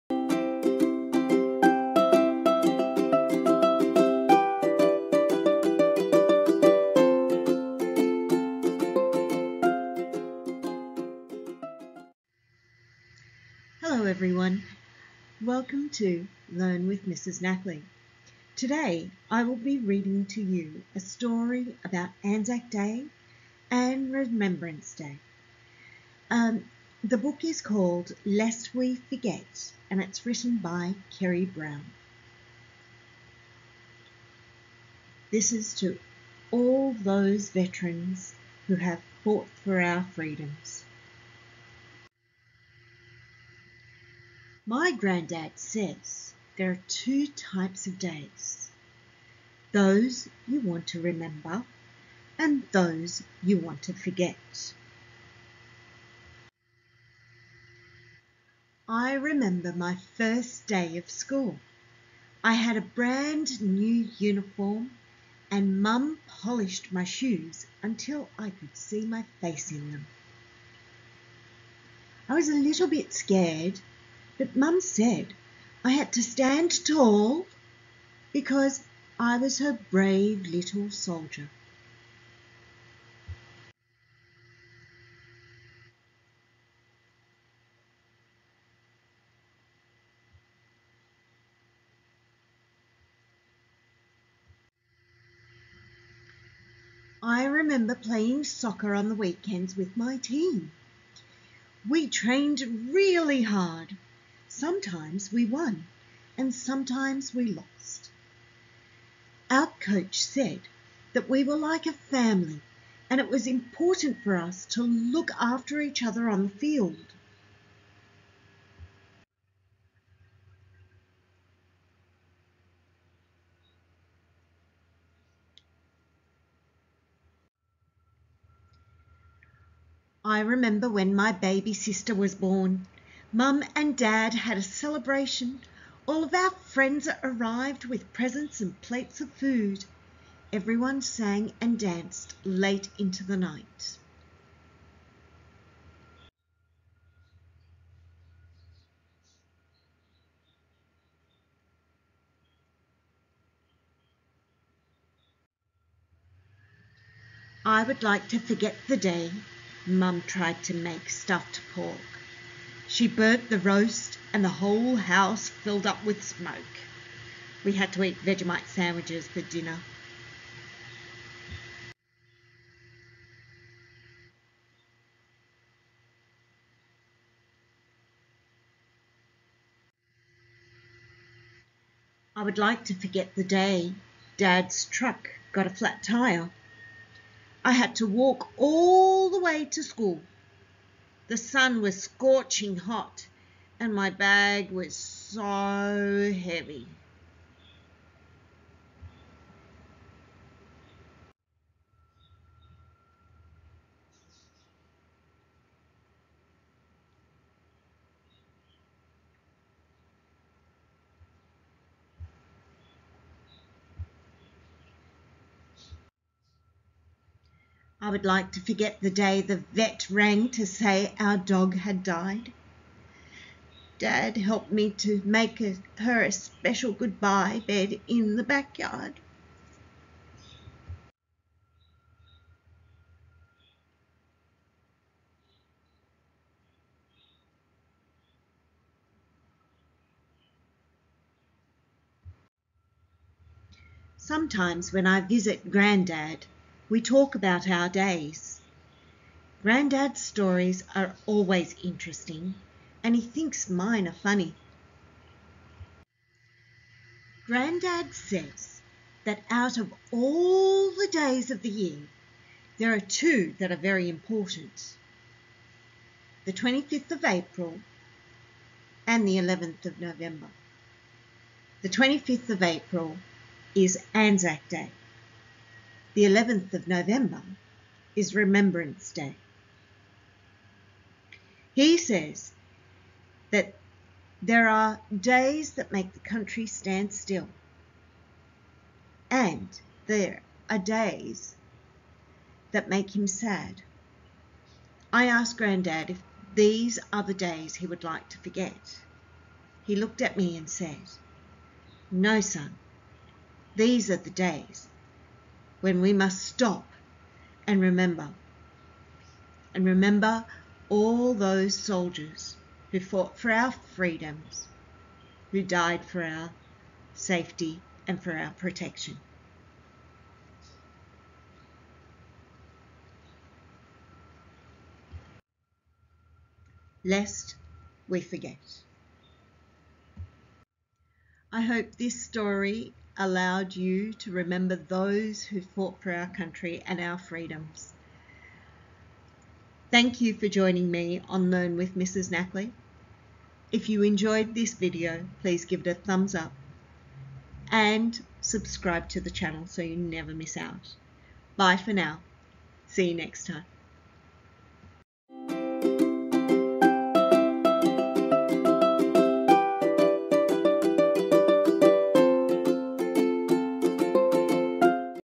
Lest We Forget - Read along with Mrs Nakhle - Story time EP22
Listen and /or read along with Mrs Nakhle as she reads 'Lest we Forget'  by Kerry Brown.

'Lest We Forget' is a conversation in which a boy and his grandfather share "days to remember and days to forget.".

It is a tribute to those who fought in wars to protect us and to those who lost their lives whilst protecting us.

This book acknowledges ANZAC Day and Remembrance Day.

Acknowledgements:
1. Author/ Illustrator - Kerry Brown / Isobel Knowles and Benjamin Portas
2. Images used Annie Spratt on Unsplash.com